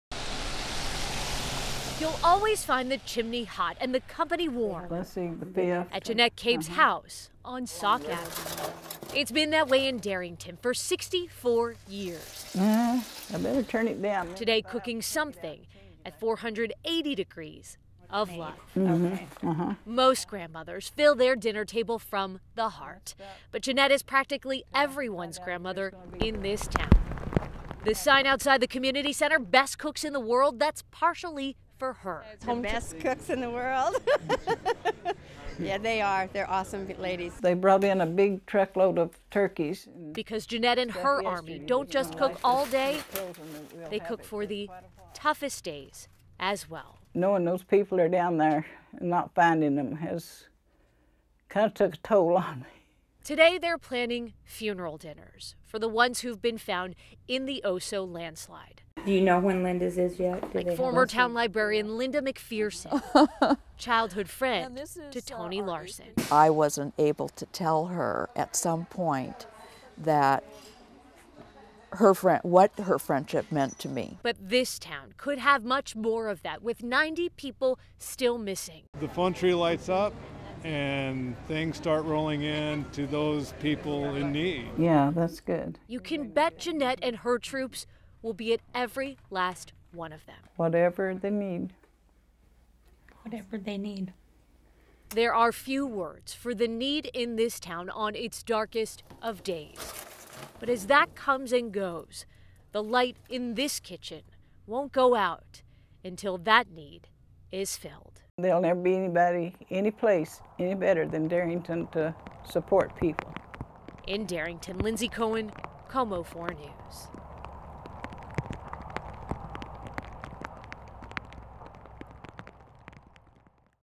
Community Strength
This is the story of a special lady in Darrington who aims to help the famalies of those who lost someone in the Oso slide.